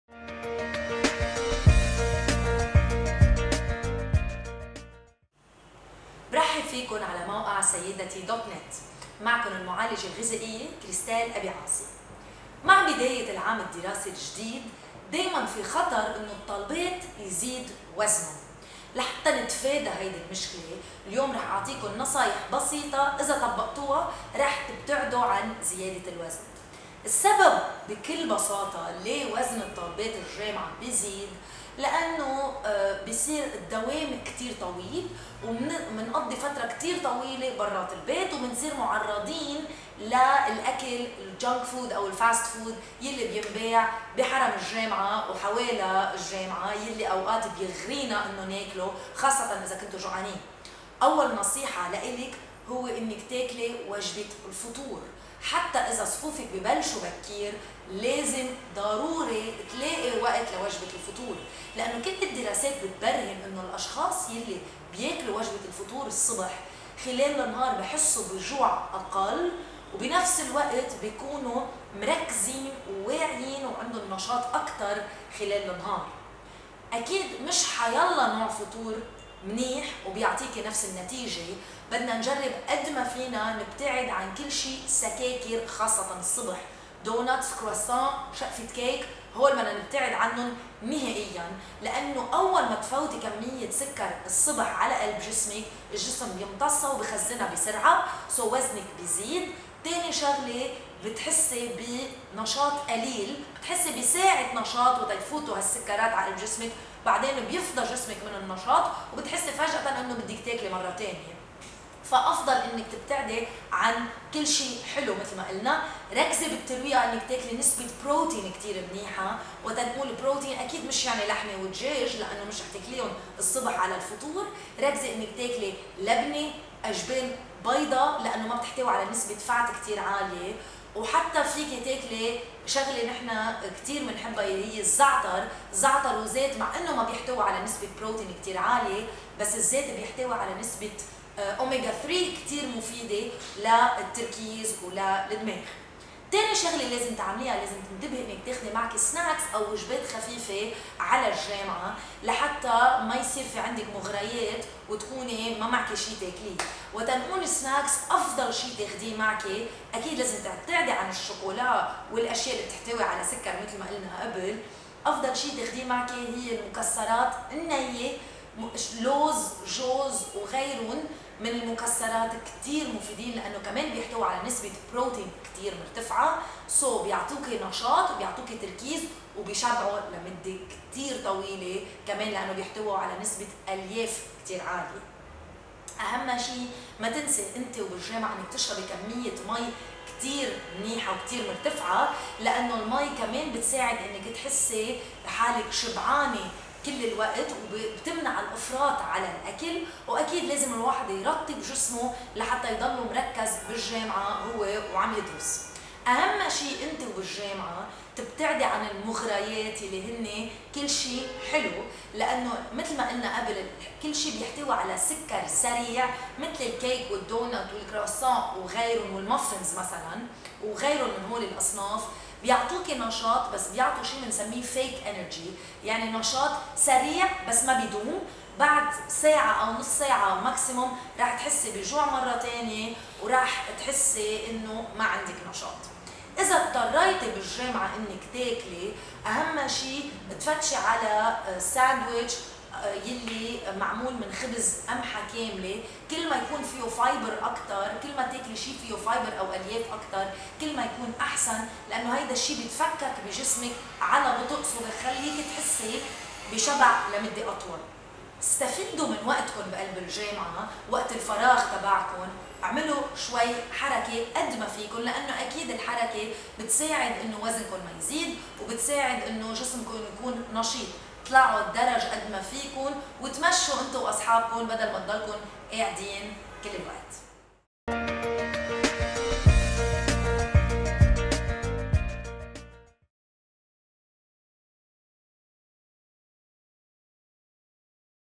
نصائح مجرّبة لرشاقة الطالبات
مع بداية العام الجامعي الجديد، ثمّة أزمة تواجه الطالبات، متمثّلة في حظوظهن المرتفعة في زيادة أوزانهنّ، بسبب قضاء جلّ أوقاتهن خارج المنزل، وبالتالي استهلاك وجبات مالحة وحلوة غير صحيّة!
ولتفادي هذه الأزمة، تُقدّم المعالجة الغذائيّة كريستال أبي عاصي، في ما يلي، أبرز النصائح الهادفة إلى الحفاظ على جسم رشيق ومتناسق، لهذه الفئة من الفتيات:

قراءة المزيد
تجنّبي أخطاء المكياج التي ترتكبها النجمات

أسوأ إطلالات النجمات بالرموش الاصطناعية